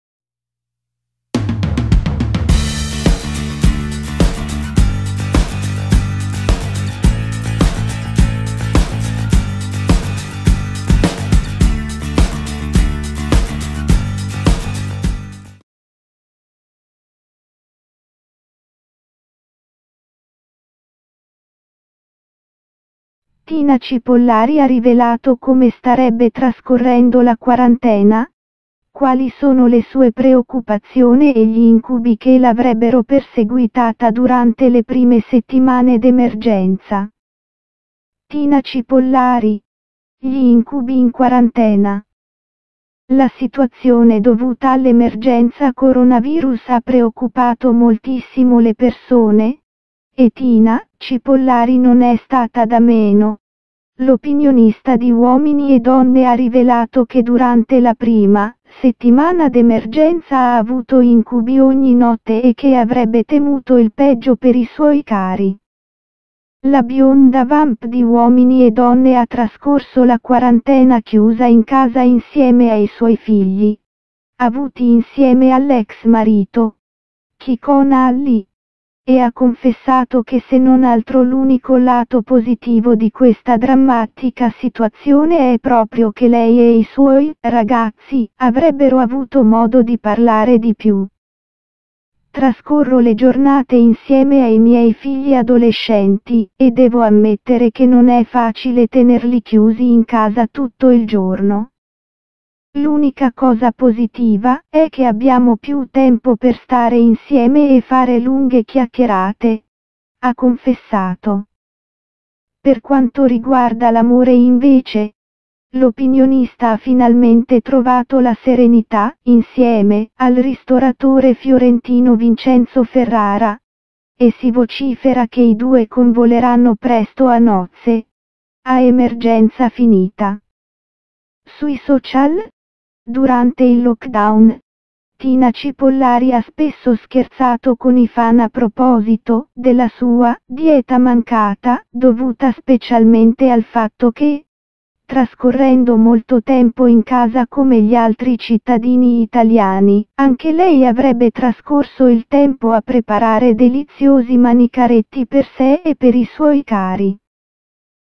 Tina Cipollari racconta degli incubi durante la quarantena wav
E adesso vi dedichiamo questo articolo dedicto a Tina CIpollari, la mitica consigliera del trono Over e Classico di Uomini e donne . E ovviamente acerrima rivale di Gemma Galgani, che insomma fra le due donne non ci sia nessun tipo di affetto ormai è cosa fin troppo risaputa. Ma non solo infatti Tina Cipollari in questa intervista ci racconta come ha passato il suoperiodo di quarantena e non sembra che l'abbia virruto al massimo...